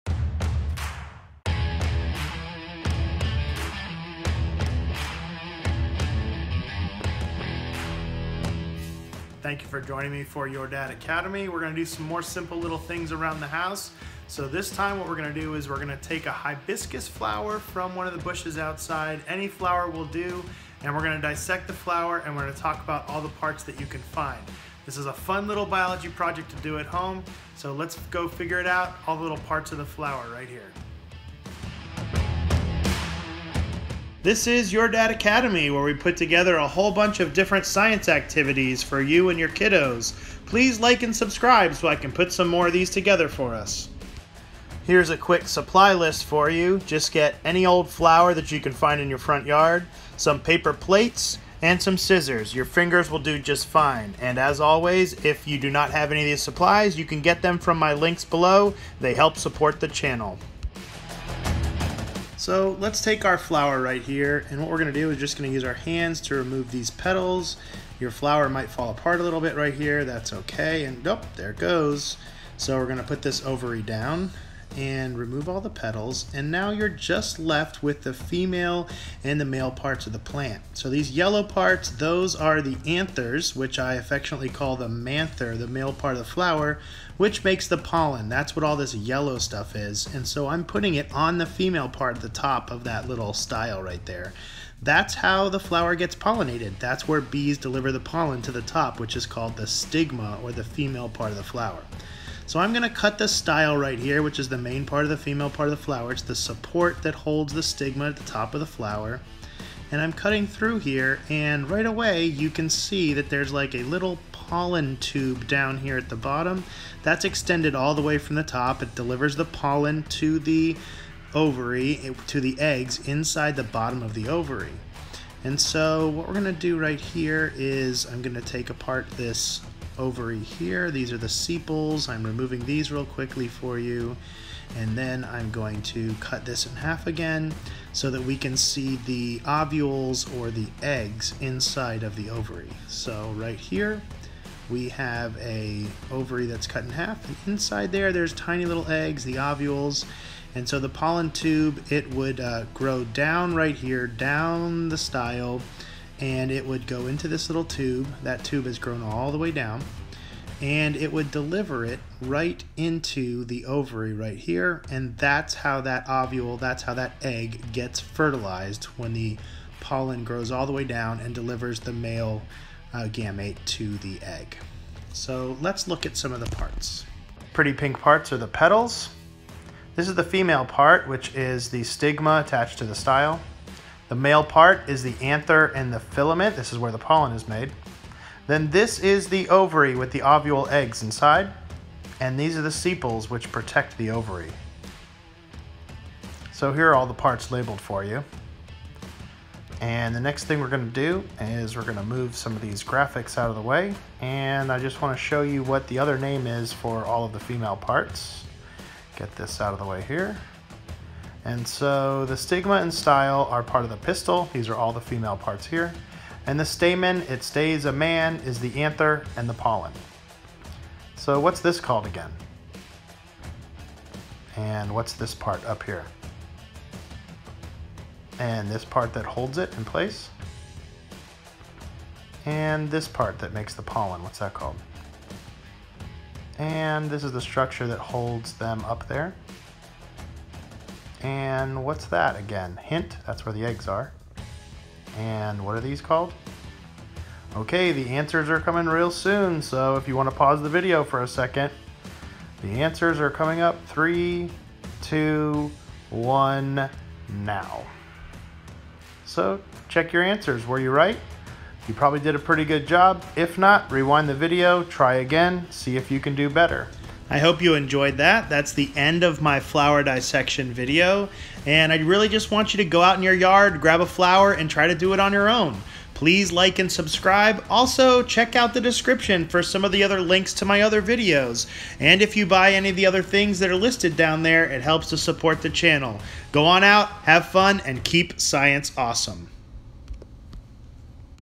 Your Dad Academy - Let’s Dissect A Flower
This is a quick and easy version of a plant dissection lab. My students love this one...and most people have flowers growing around their house. so grab one and start dissecting. There are all sorts of varieties of flowers...this one is a hibiscus flower...Google the flower that you have to see how the parts are different.

Paper Plates